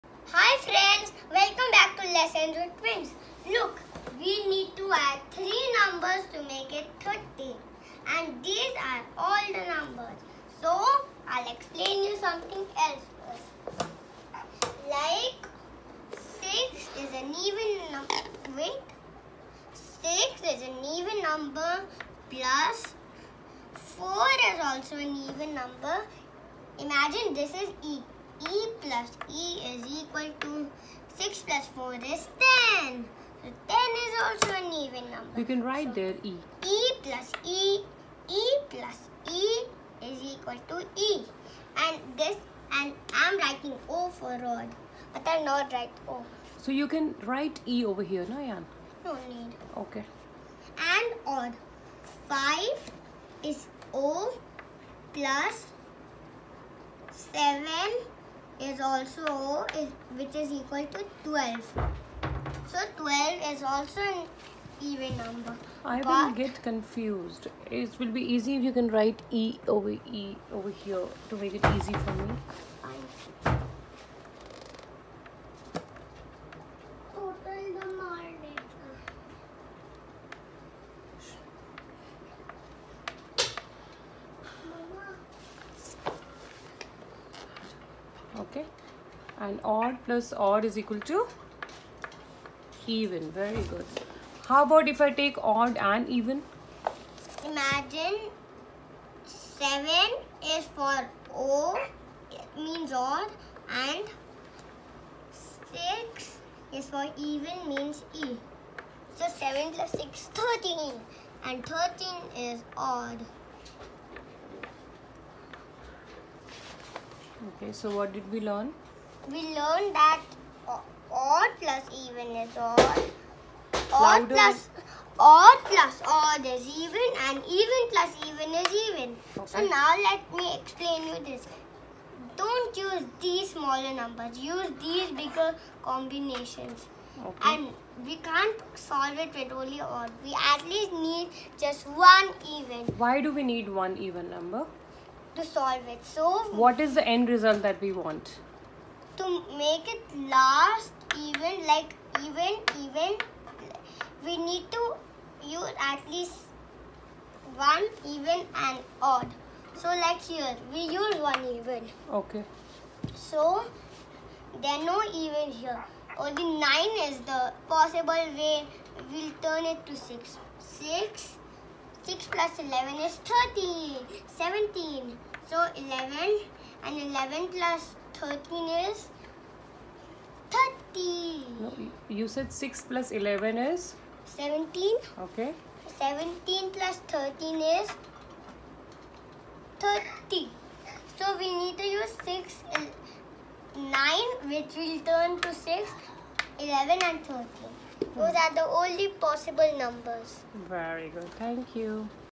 Which 3 numbers would add upto 30
Let’s learn how to solve the puzzle to add 3 numbers to make a total of 30.